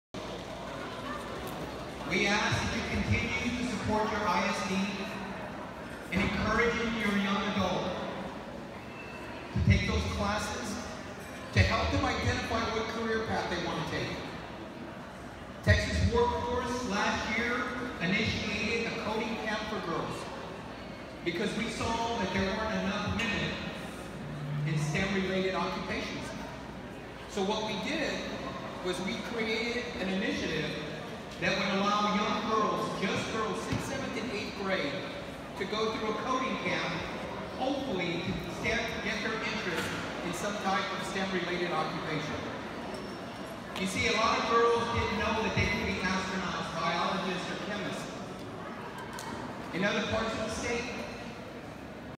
TWC Commissioner Julian Alvarez Speaks to Students at Round Rock ISD Career Exploration Fair
“Texas Workforce last year initiated a coding camp for girls because we saw that there weren’t enough women in STEM-related occupations…A lot of girls didn’t know that they could be astronauts, biologists and chemists.” TWC Commissioner Julian Alvarez spoke to students and parents last night at the Round Rock ISD Career Exploration Fair, stressing the importance of helping our youth identify suitable career paths. We can help! Find TWC-supported programs and talk to LMCI Career Education experts